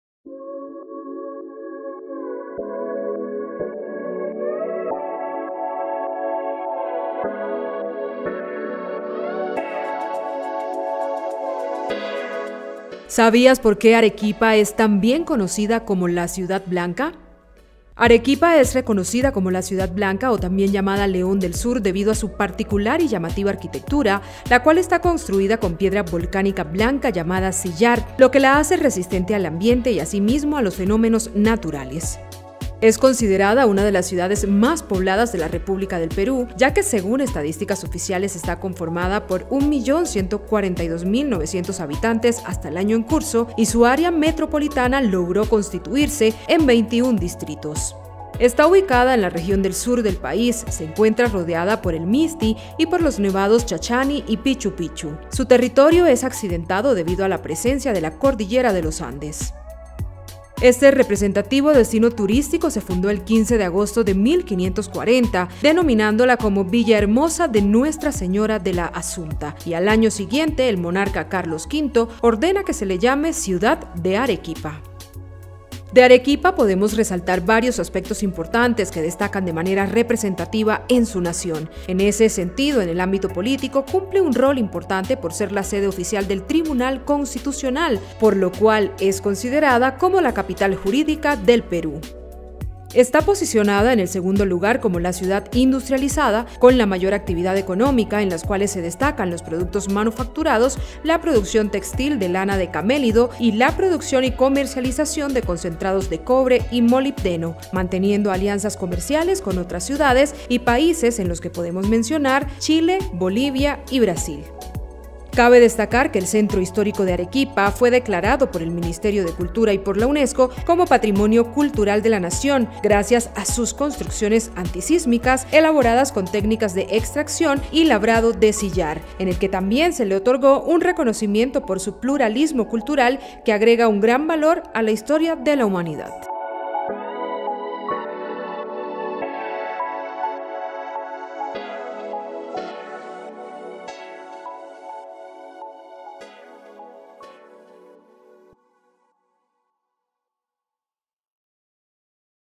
¿Sabes por qué Arequipa es también conocida como la ciudad Blanca?
Política, cultura, historia y datos curiosos; giran alrededor de la ciudad de Arequipa, ubicada en Perú y considerada en la actualidad una de las ciudades más grandes de esta república.

En el año 1540 su nombre era totalmente distinto con el que hoy es conocida mundialmente. ¿Quieres saber cuál era ese nombre?, pues te invitamos a ver la nota completa.